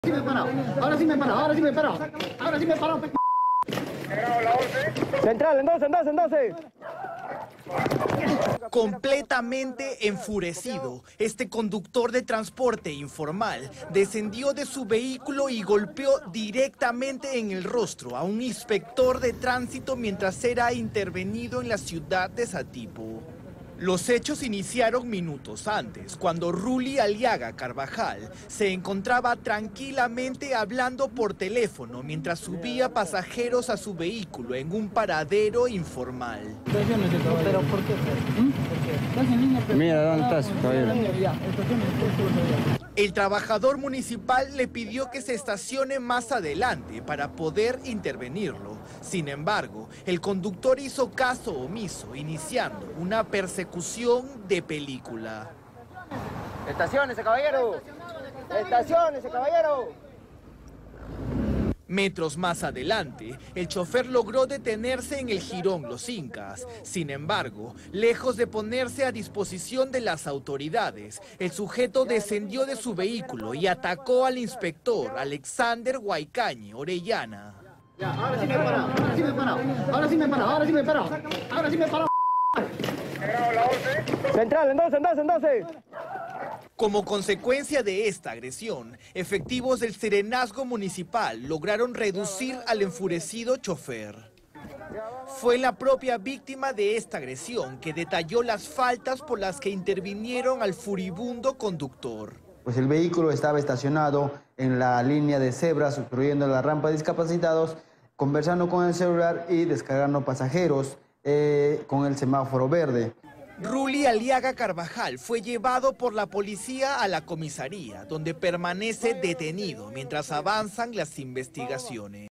Satipo: chofer que golpeó a inspector de tránsito acabó en la comisaría
Un conductor informal atacó a golpes a un inspector de tránsito durante una intervención de rutina en la ciudad de Satipo. El agresor fue identificado como Ruly Aliaga Carbajal, quien fue sorprendido bajando pasajeros en una zona prohibida.

Lejos de reconocer su infracción, el sujeto intentó darse a la fuga, por lo que la autoridad municipal inició una persecución. Metros más adelante, el chofer se bajó de su unidad y arremetió contra el efectivo Alexander Huaycañé Orellana.

En ese momento, agentes del Serenazgo se hicieron presentes en el lugar e intervinieron a Ruly Aliaga Carbajal, quien fue trasladado a la comisaría del sector, donde será denunciado por los delitos de resistencia y violencia a la autoridad.

INSPECTOR SE PRONUNCIA

“El vehículo estaba estacionado en la línea de cebra, obstruyendo la rampa de discapacitados, conversando con el celular y bajando pasajeros con el semáforo en verde”, sostuvo el inspector municipal en diálogo con periodistas locales.